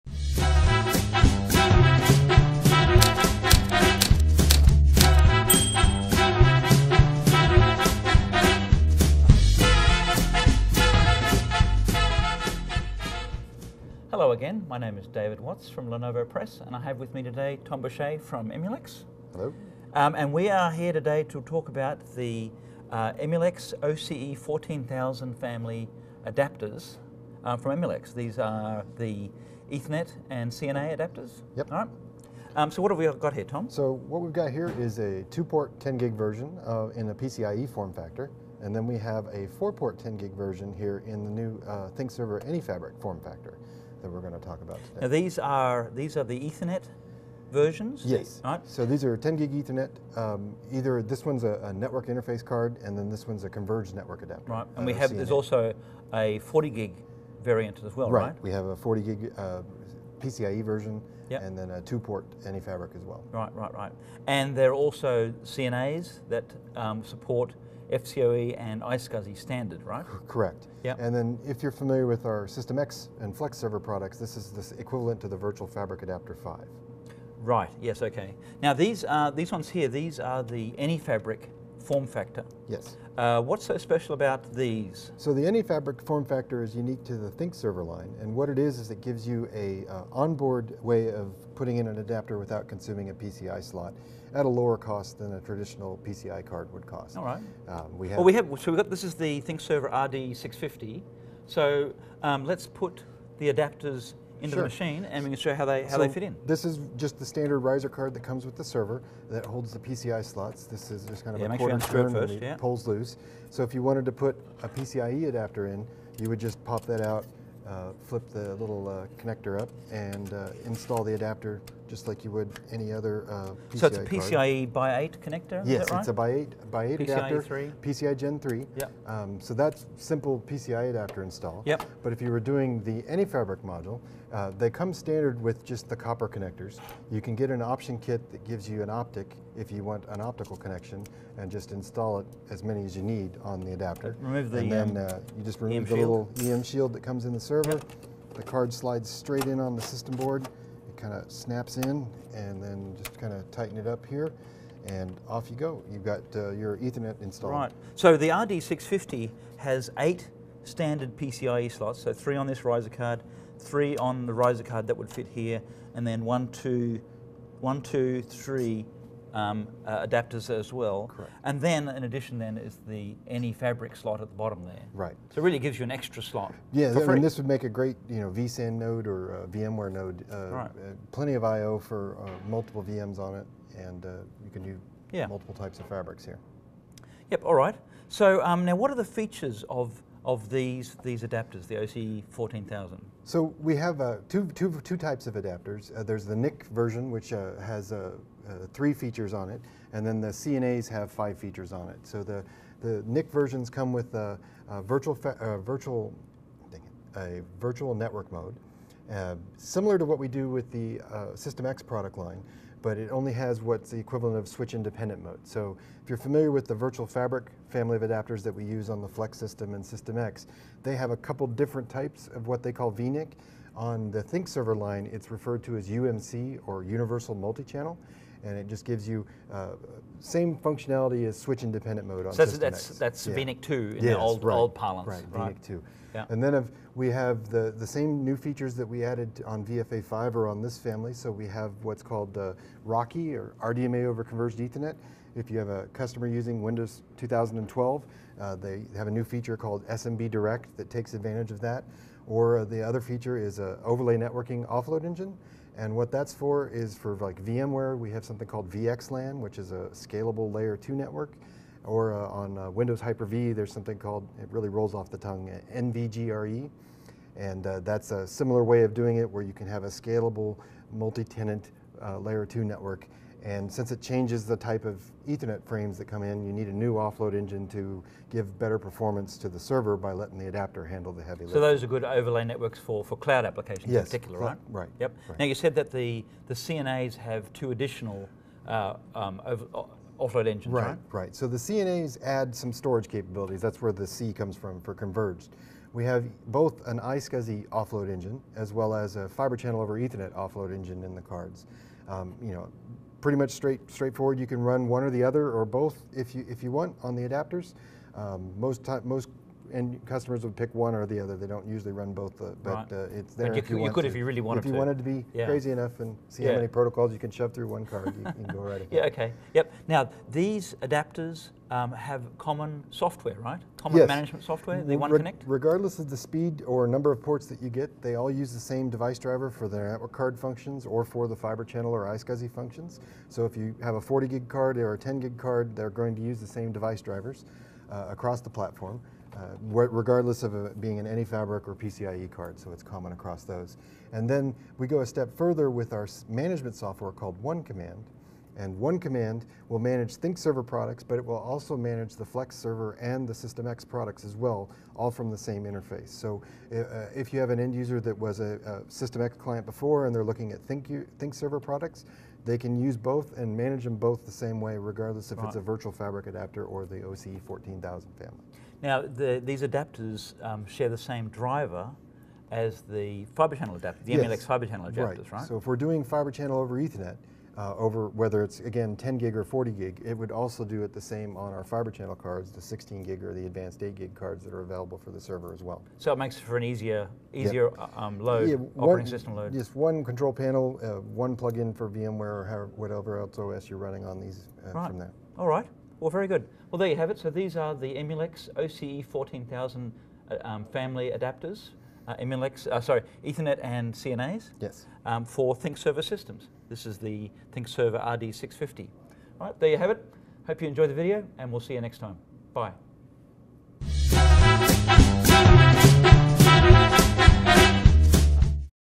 Emulex OCe14000 Networking & Converged Networking Adapters for ThinkServer
Learn about the Emulex OCe14000 family of NICs and CNAs with this walk-through video by David Watts (Lenovo Press) and Tom Boucher (Emulex). For more information about the adapters, see the Lenovo Press Product Guide Video production by Keith Blatz and Joe Goldston.